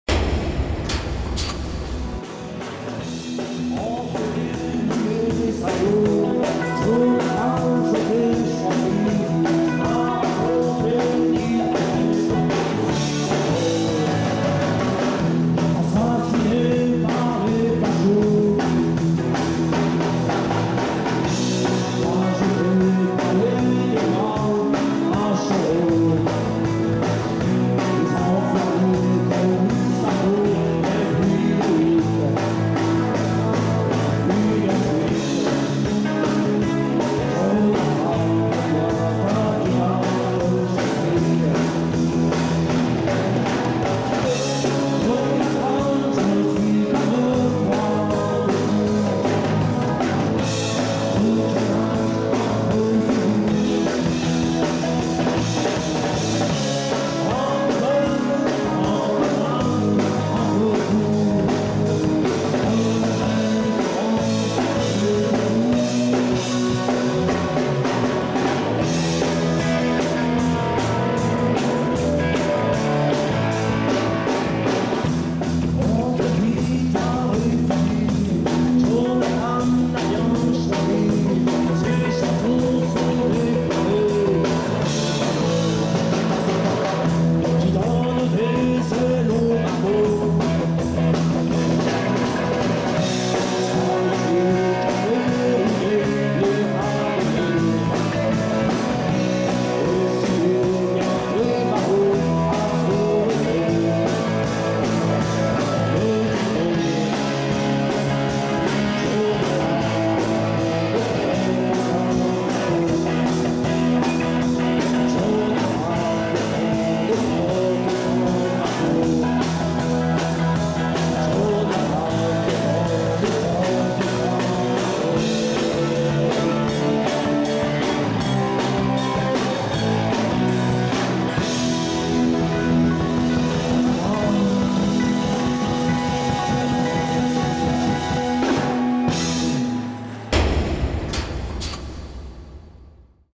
Forum des associations de Rouen de Normandie 1ère Edition le 5 novembre 1994 à Rouen vidéo3/5
Les Clenches au Forum des associations de Rouen de Normandie 1ère Edition le 5 novembre 1994 à Rouen organisé par l'association ASPRA